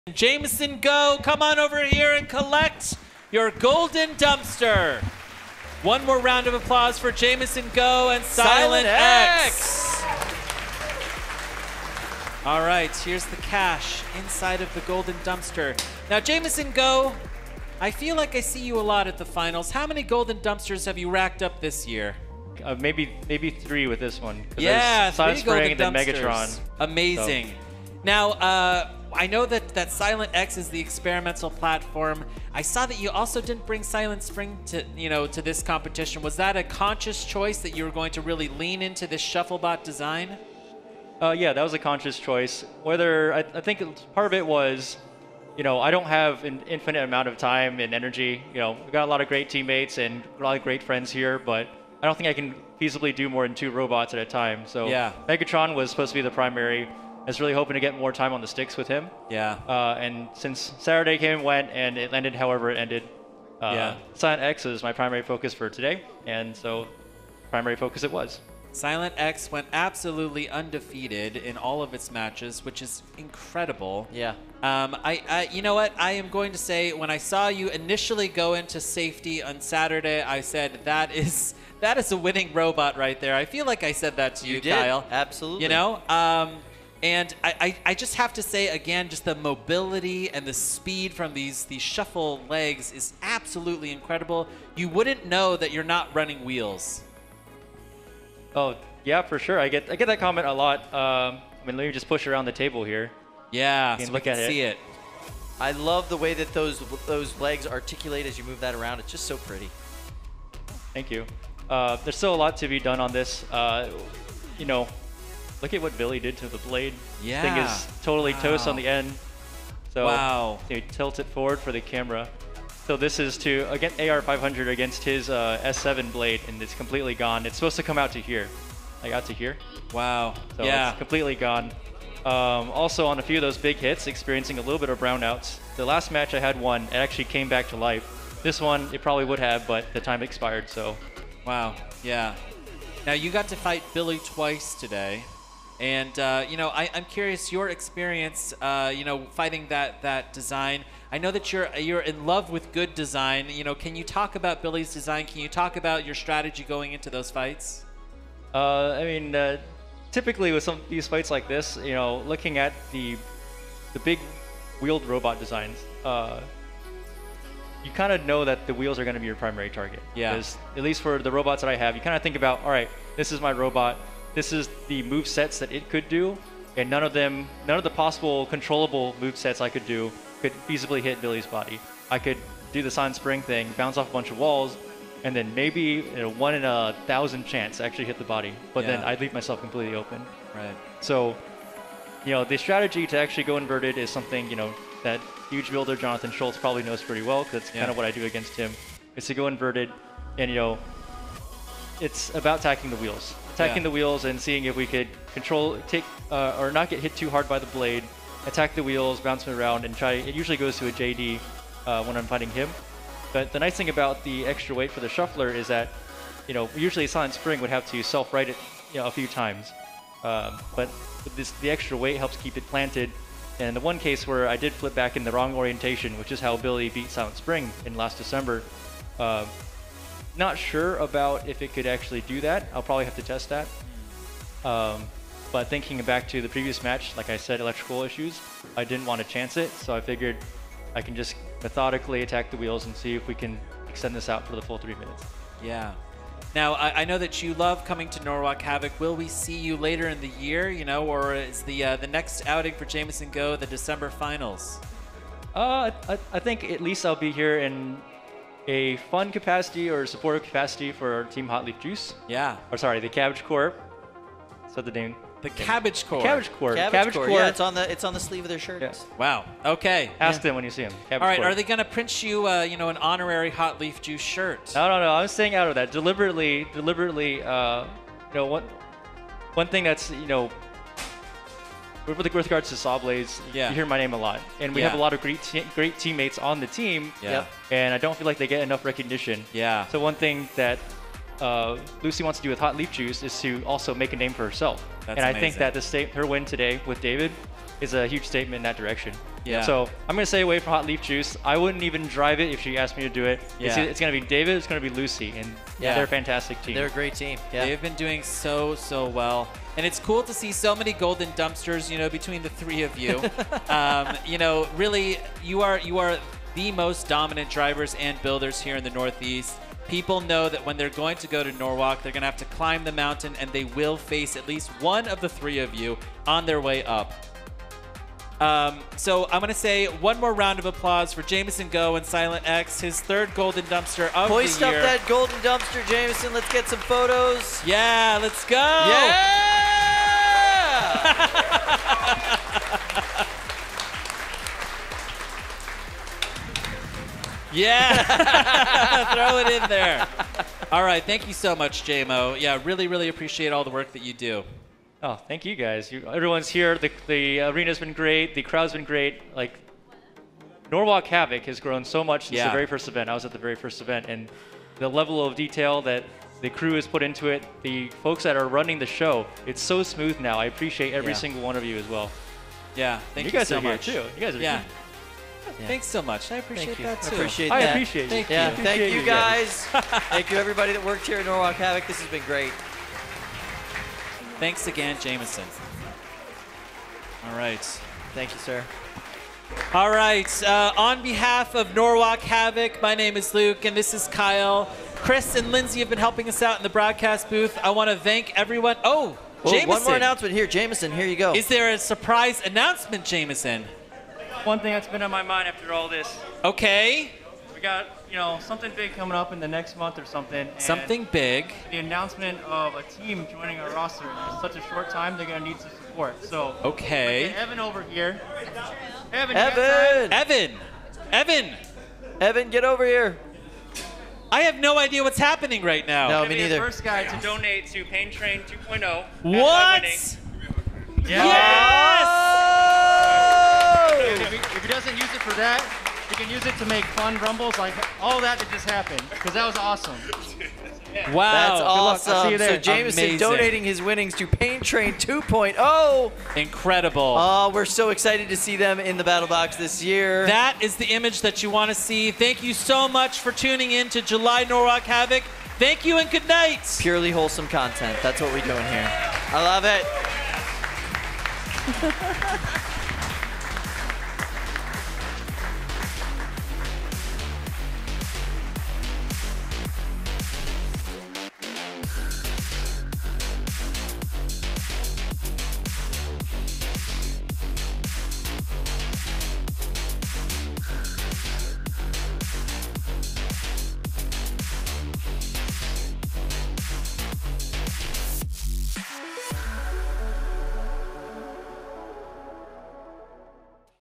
NHRL July 2021 3lb Winner's Interview
Jamison Go tells us about his bot, Silent X, as he adds another Golden Dumpster to his collection!